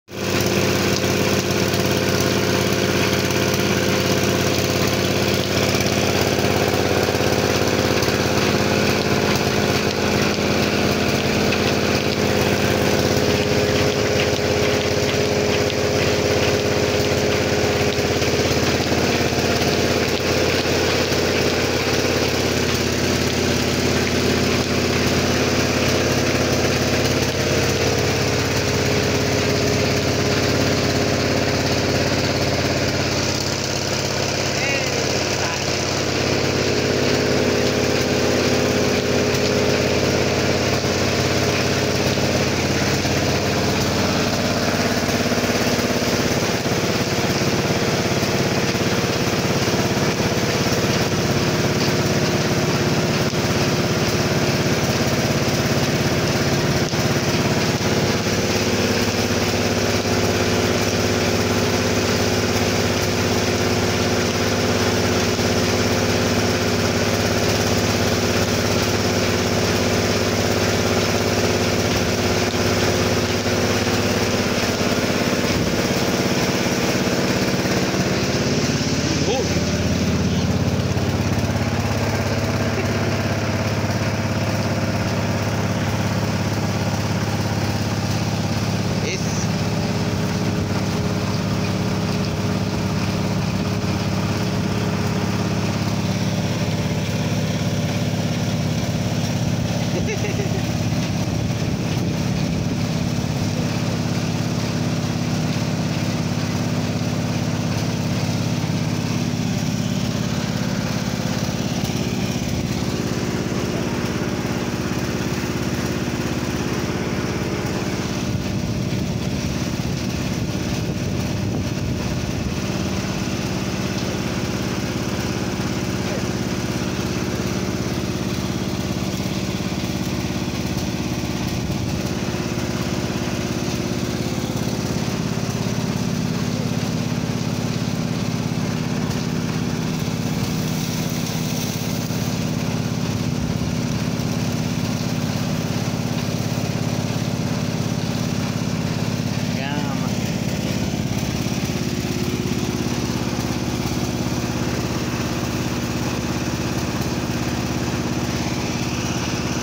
VIDA NO ENGENHO
Engenho de cana de açúcar no interior do Ceará.